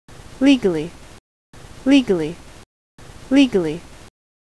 How to Say or Pronounce Legally
This video shows you how to say or pronounce Legally.

How would you say Legally?